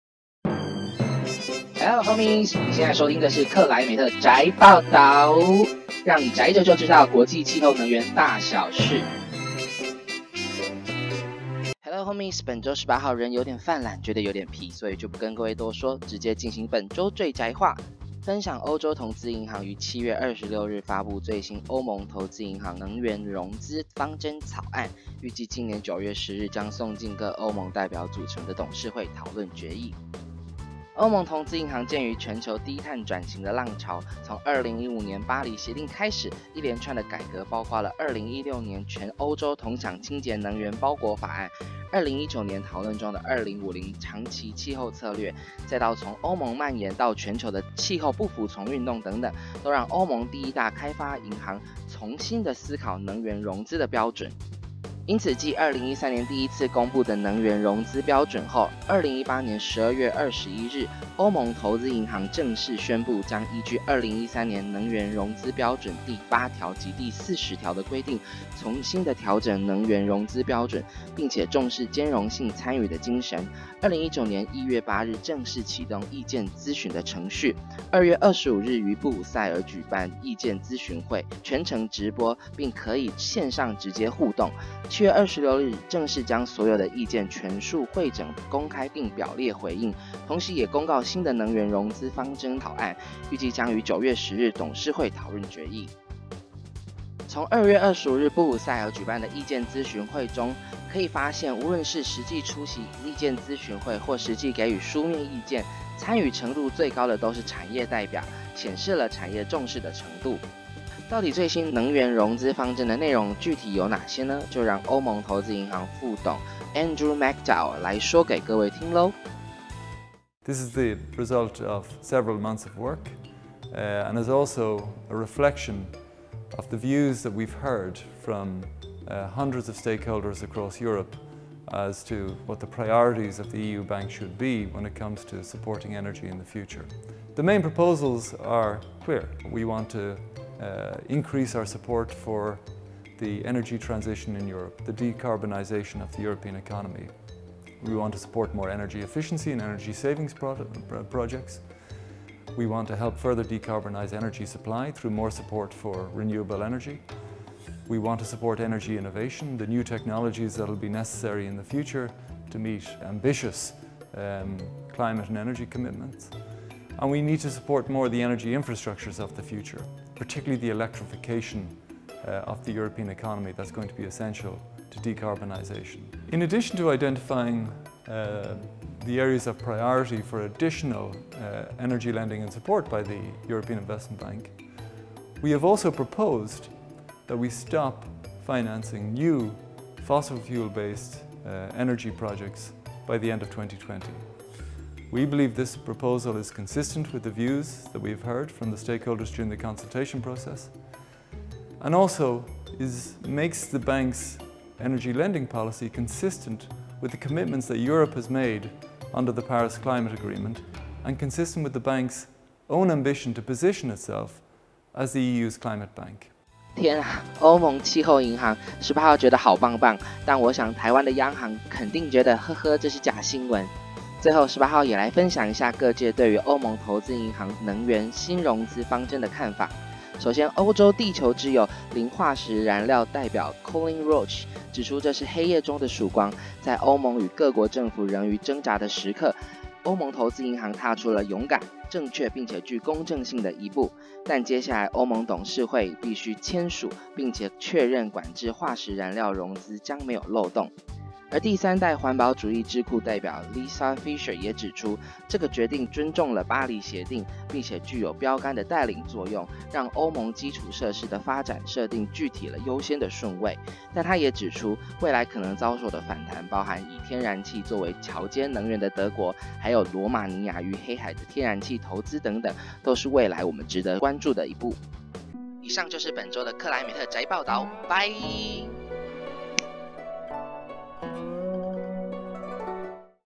【台大風險中心】克萊美特宅報導_EP08
【克萊美特宅報導Climate Homie_EP08】
讓你宅著就知道國際氣候能源大小事 快速瀏覽本期重點
👌本週最宅話：歐洲投資銀行(EIB)提出2019能源新融資方針草案！
★低碳轉型的浪潮下歐洲金融業必須做出新的因應對策
★研議新的能源融資標準
★更兼容性的納入大範圍的利害關係人評估
★來聽聽EIB副董 Andrew McDowell 怎麼說～